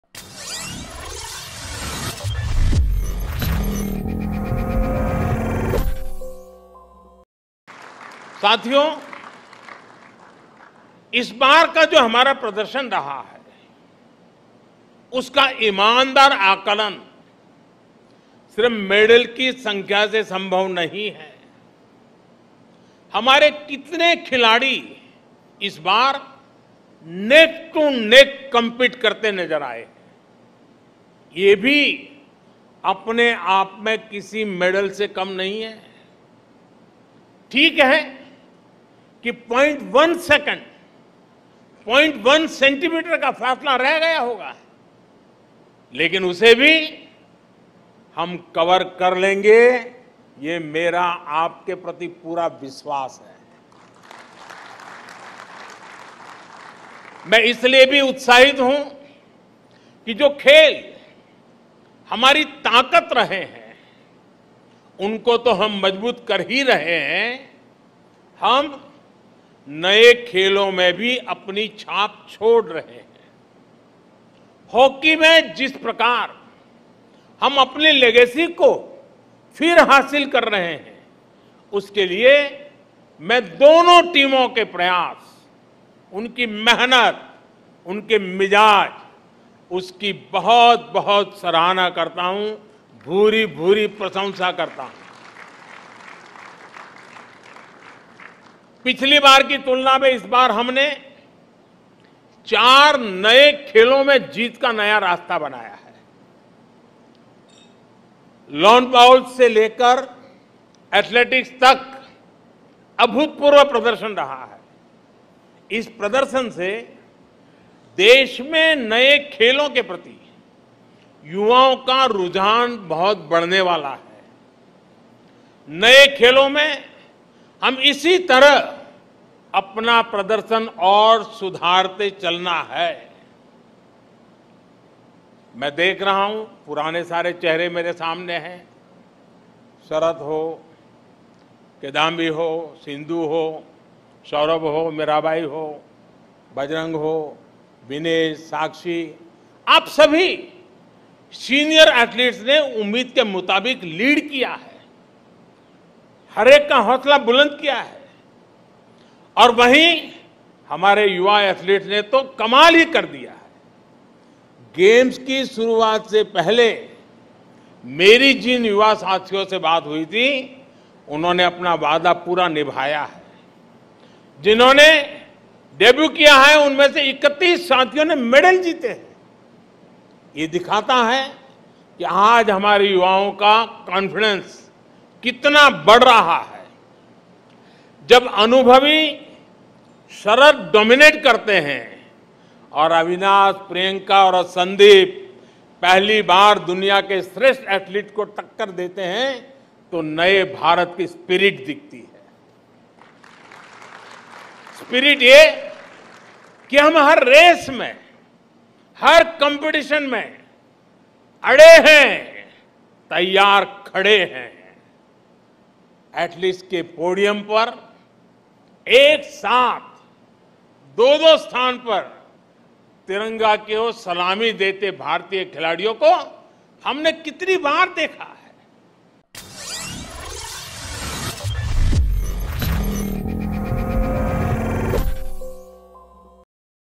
CWG 2022: PM Modi ने बता दिया आखिर क्यों उन्हें खिलाड़ियों पर है इतना भरोसा
Debutants at CWG 2022 won 31 medals, this shows their confidence: PM
Producer: Umar